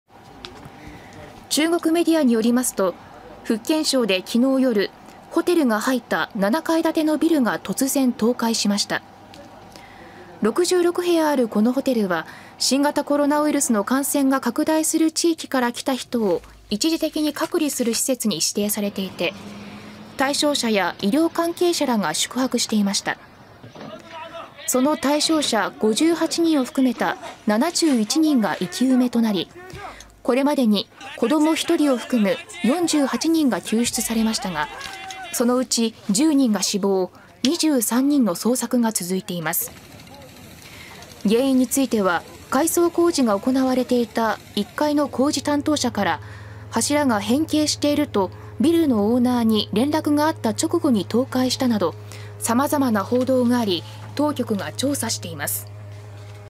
中国のホテル倒壊 死者は10人に 23人の捜索続く(20/03/08)
中国で新型コロナウイルスの隔離施設に指定されていたホテルが倒壊して71人が生き埋めとなり、これまでに10人が死亡、23人が閉じ込められたままになっています。

　中国メディアによりますと、福建省で7日夜にホテルが入った7階建てのビルが突然、倒壊しました。66部屋あるこのホテルは新型コロナウイルスの感染が拡大する地域から来た人を一時的に隔離する施設に指定されていて、対象者や医療関係者らが宿泊していました。その対象者58人を含めた71人が生き埋めとなり、これまでに子ども1人を含む48人が救出されましたが、そのうち10人が死亡、23人の捜索が続いています。原因については改装工事が行われていた1階の工事担当者から柱が変形しているとビルのオーナーに連絡があった直後に倒壊したなど様々な報道があり、当局が調査しています。